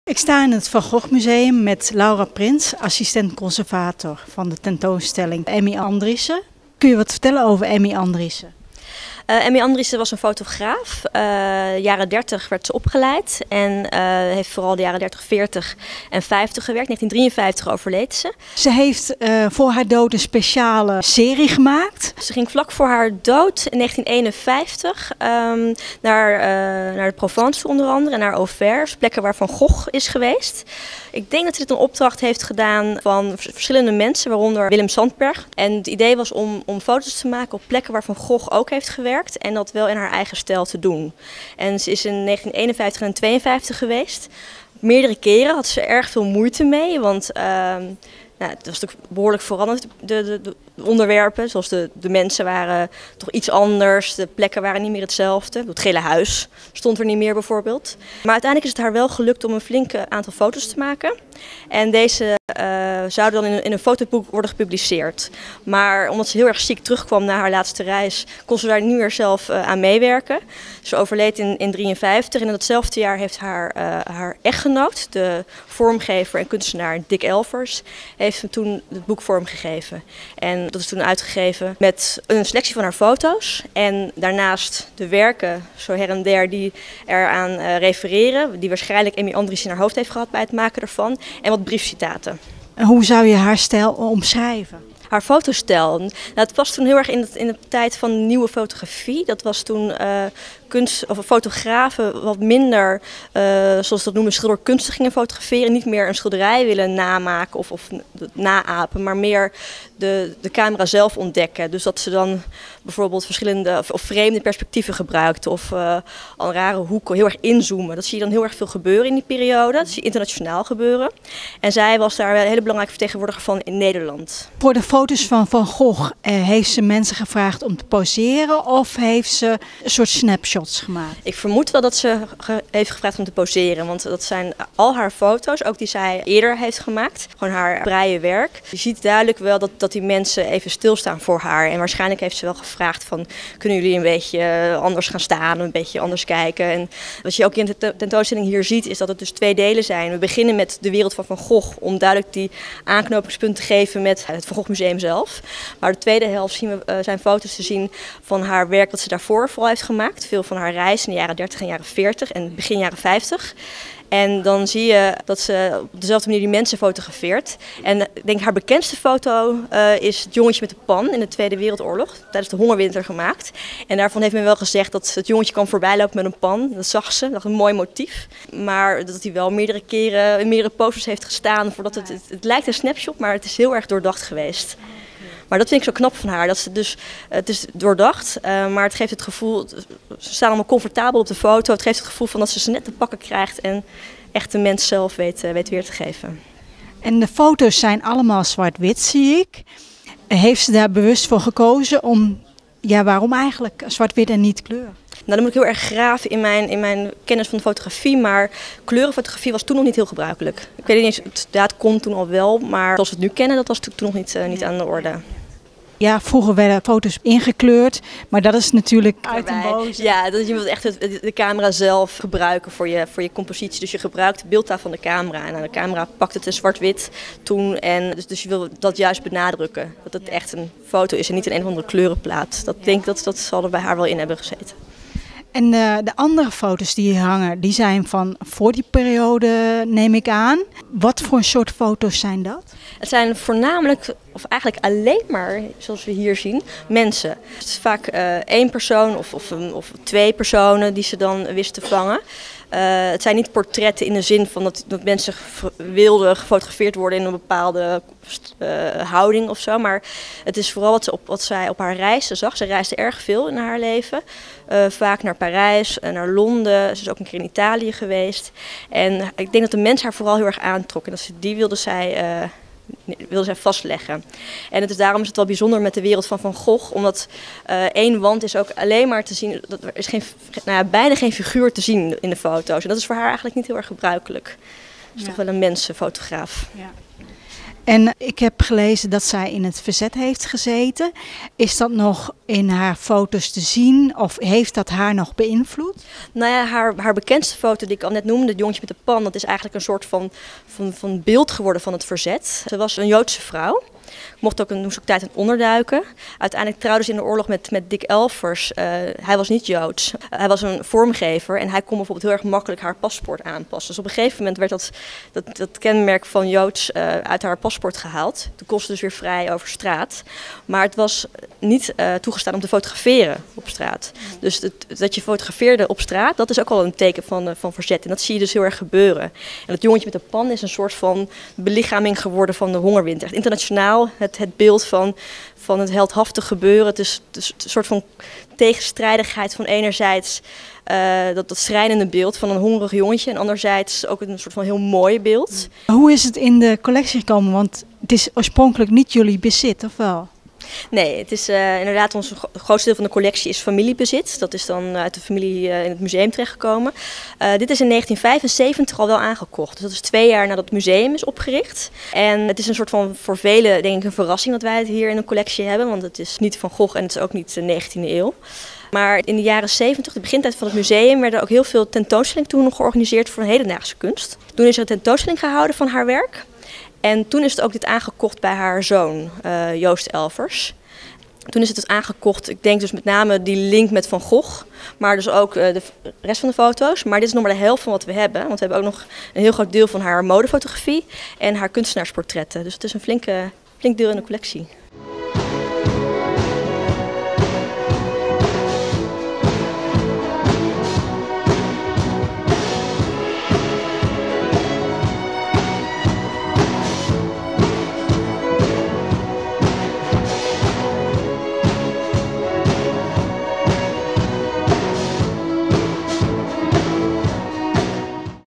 Van Gogh Museum - Foto's van Emmy Andriesse
Tentoonstelling in het Van Gogh Museum
ihkv 100e geboortejaar van Emmy Andriesse.
Interview met Laura Prins.
Amsterdam 2014.

Copyright Ratatouille TV